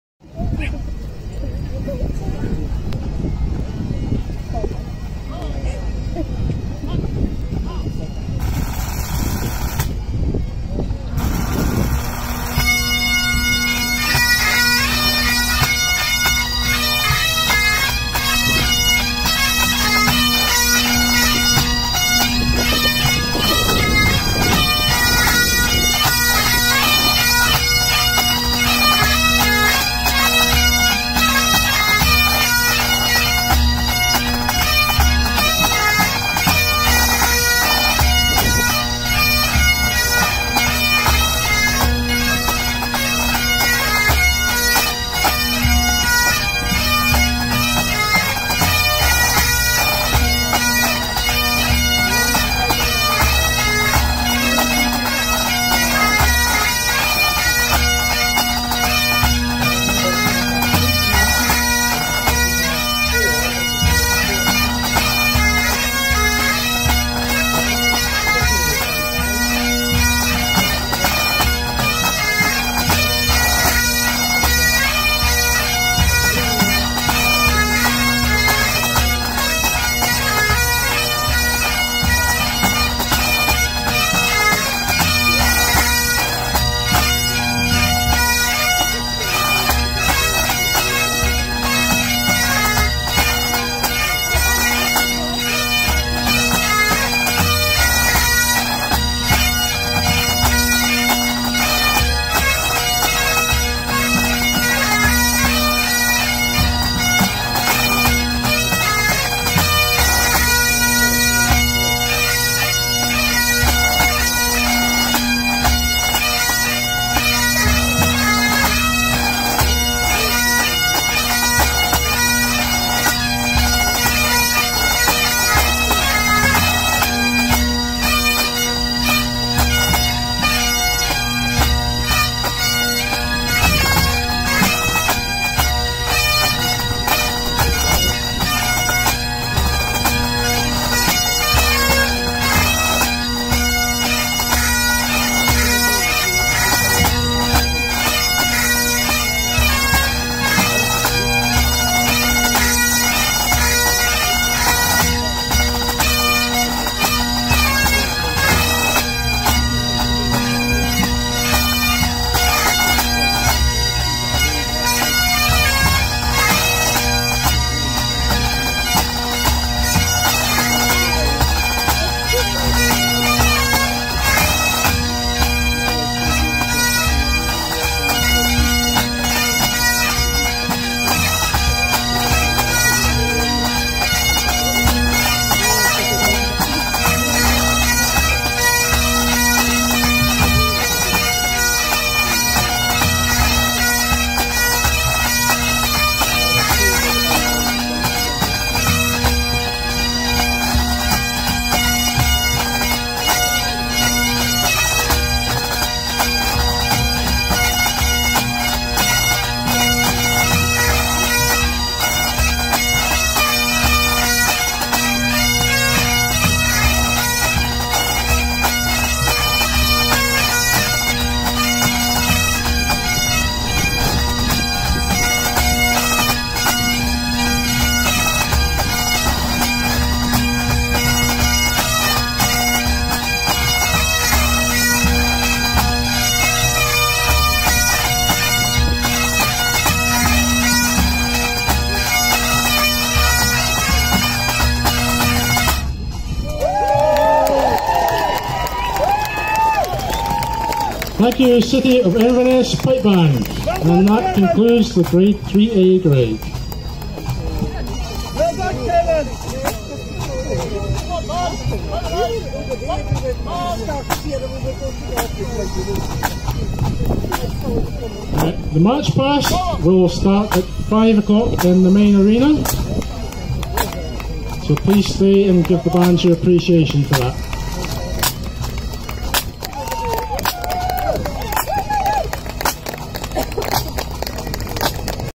City of Inverness Pipe Band
European pipe band championships in Inverness 2019